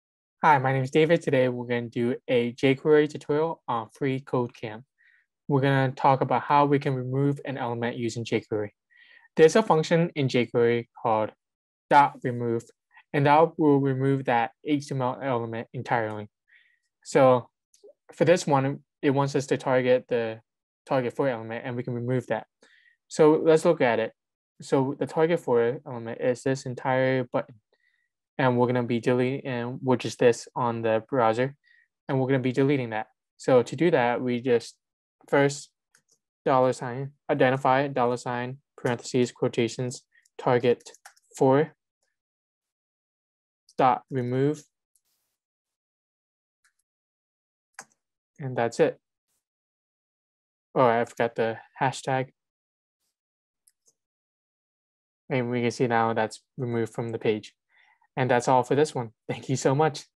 Remove an Element Using jQuery (jQuery) freeCodeCamp Tutorial
Certification: Front End Development Library
Course: jQuery
Lesson: Remove an Element Using jQuery
freeCodeCamp tutorial

In this lesson, we learn how to remove an element in jQuery using the .remove() function.